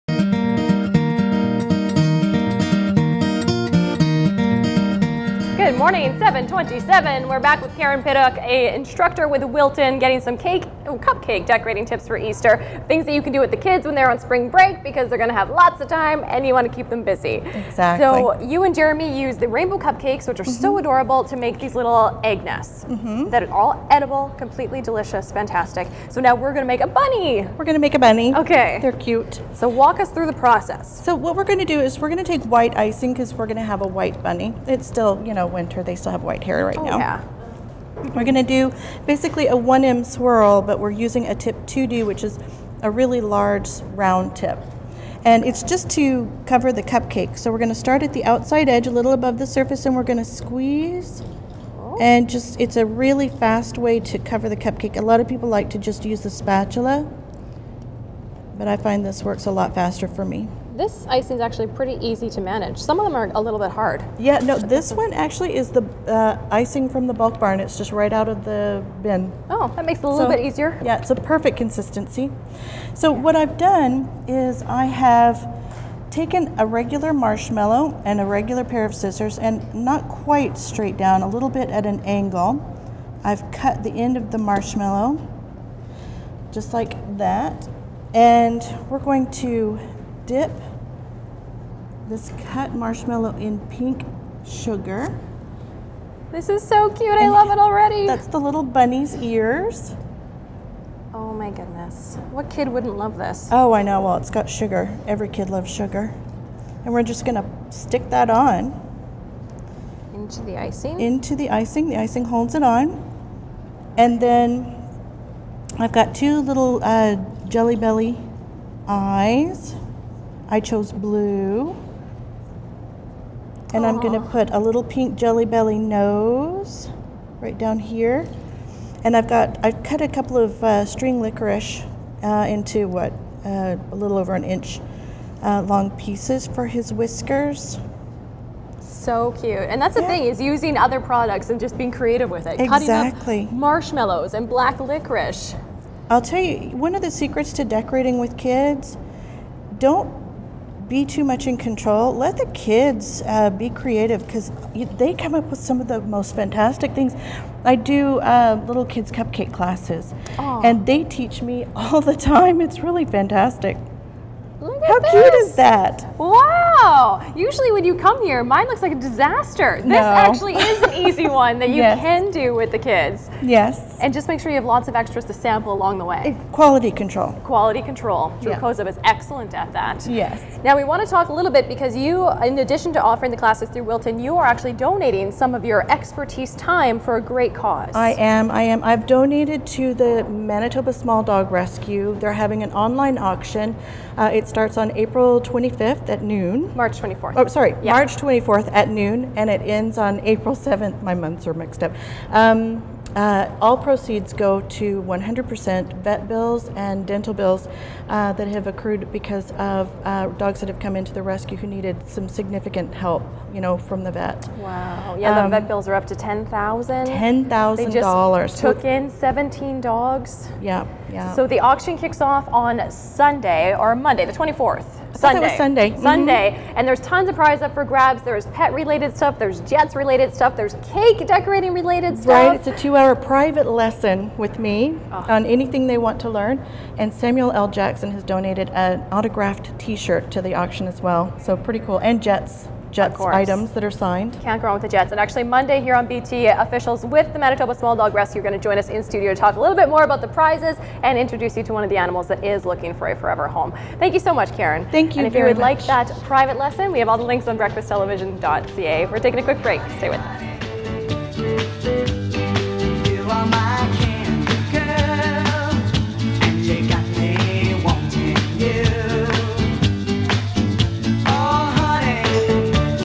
Easter Cupcakes II
Karen Pittock from Wilton Cakes makes cute bunny cupcakes for Easter!